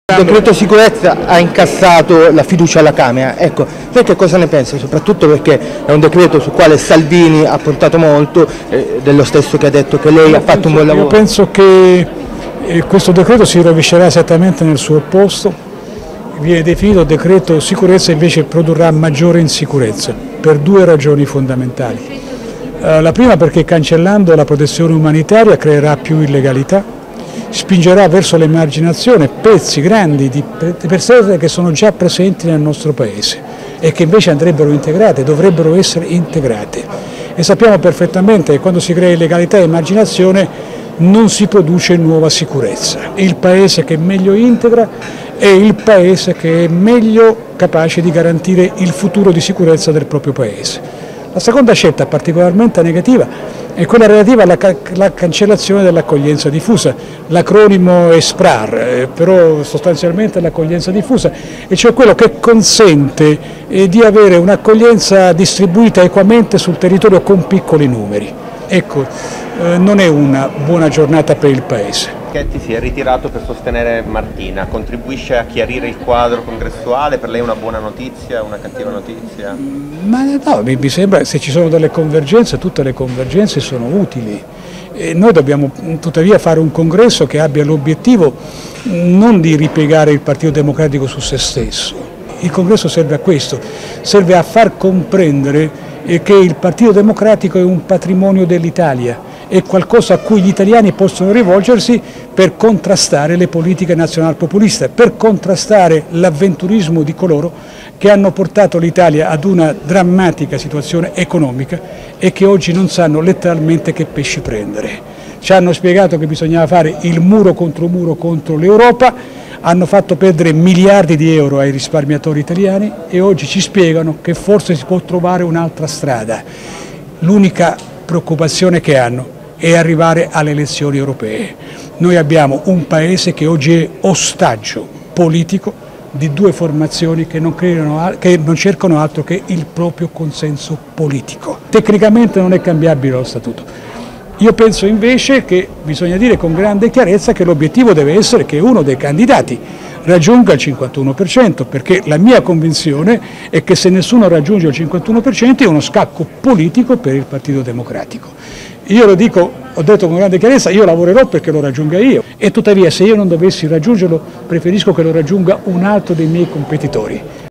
Sicurezza, Minniti: "Questo decreto si rovescerà nel suo opposto"
Marco Minniti commenta la fiducia sul Dl sicurezza incassata dal Governo. Lo fa a margine della presentazione del suo ultimo libro, a Bologna. Secondo l'ex ministro degli Interni questo decreto finirà per creare maggiore insicurezza. "La cancellazione della protezione umanitaria avrà come conseguenza la creazione di maggiore illegalità ed emarginazione", spiega. .  .L'ex ministro torna poi a commentare la corsa alla segreteria del Pd. "L'obbiettivo deve essere che uno dei candidati raggiunga il 51 per cento", afferma. "Lavorerò per raggiungerlo, se non dovessi riuscirci preferisco che lo raggiunga un altro dei miei avversari piuttosto che nessuno". .  .Video di Valerio Lo Muzio